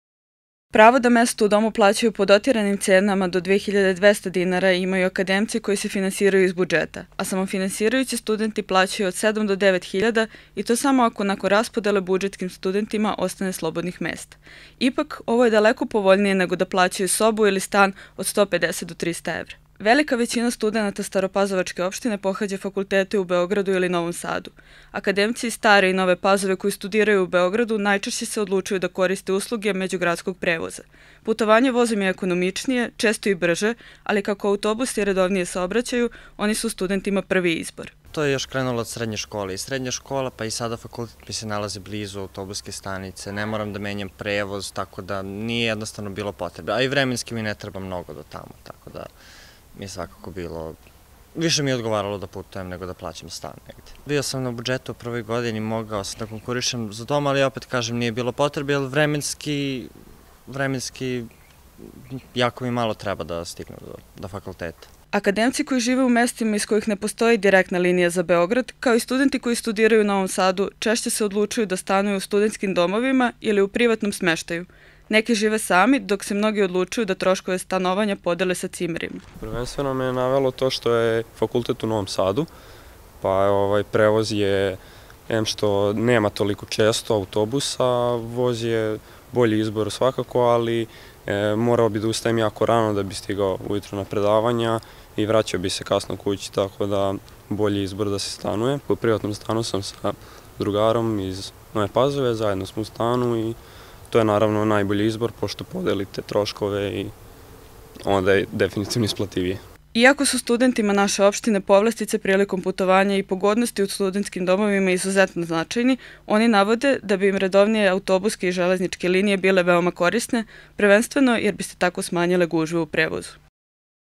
Stan, studentski dom ili putovanje?
Studentima je u Srbiji na raspolaganju devet studentskih centara sa oko 17.500 mesta, od čega više od 10.000 u Beogradu. Međutim, kako se na konkurs svake godine prijavi oko 40.000 akademaca, čini se da su ovi kapaciteti nedovoljni. Zbog toga su mnogi od studenata koji stanuju na teritoriji naše opštine odlučili da potraže smeštaj u privatnim stanovima ili da do fakulteta svakodnevno putuju autobusom ili vozom.